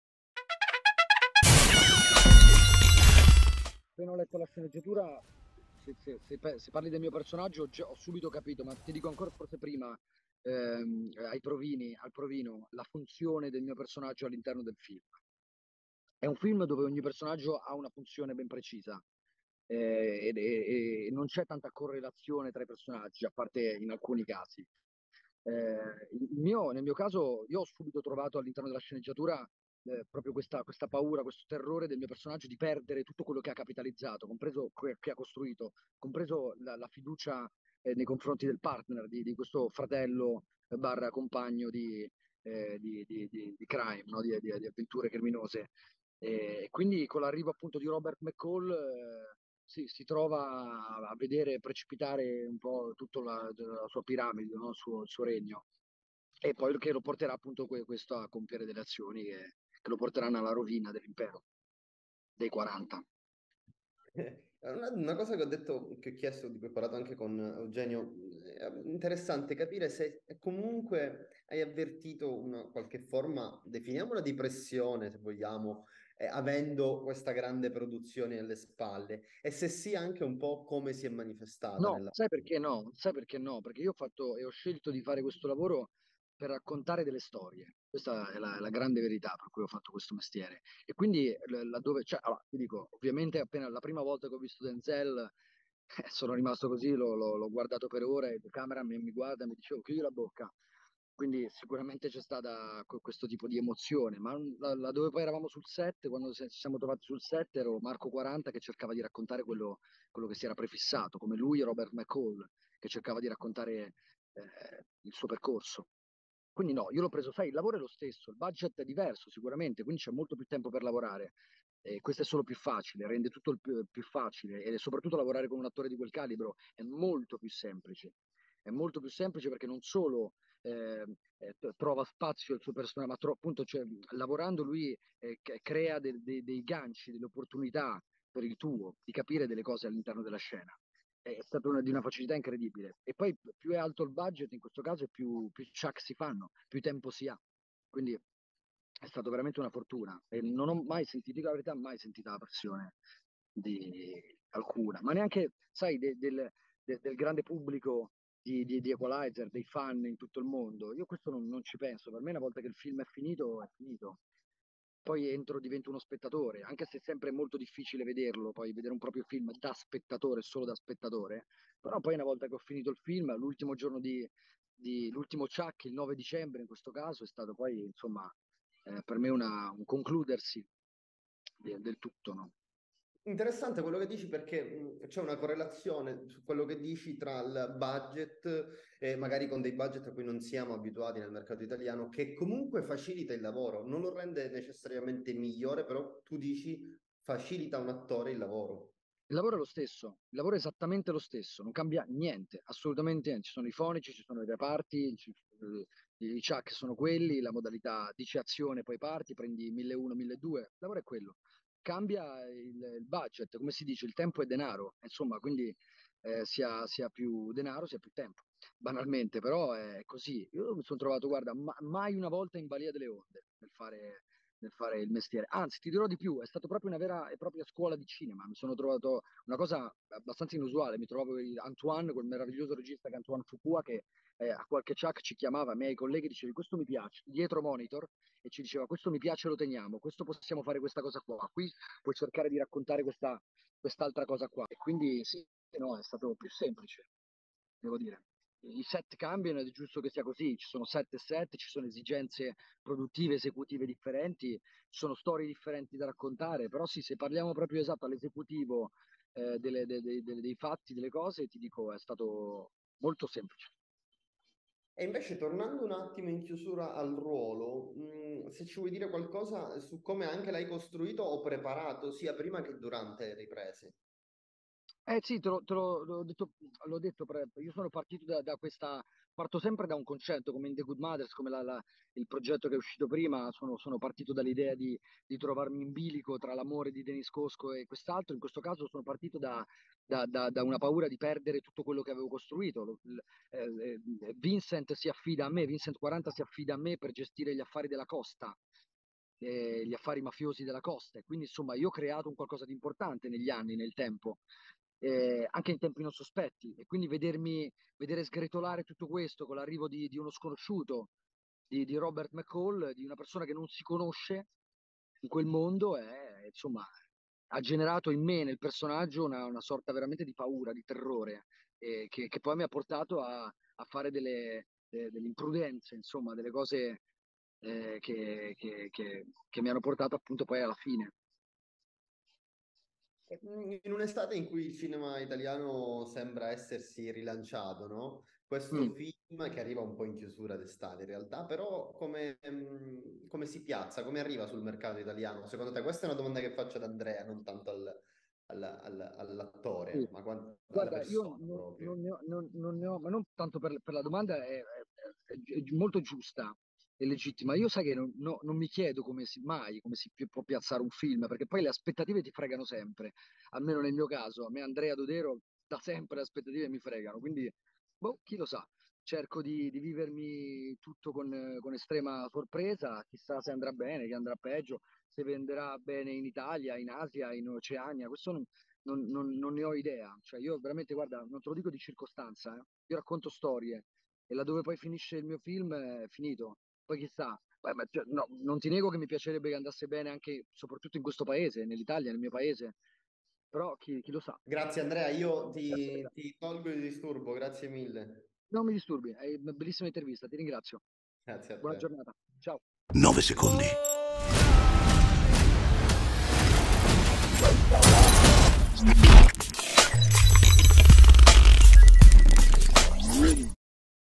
Intervista ad Andrea Dodero | The Equalizer 3 | HOT CORN
Hot Corn Tv | Andrea Dodero, ospite della redazione di Hot Corn, ha raccontato la sua esperienza sul set di  "The Equalizer 3" diretto da Antoine Fuqua, con Denzel Washington e Chloë Grace Moretz.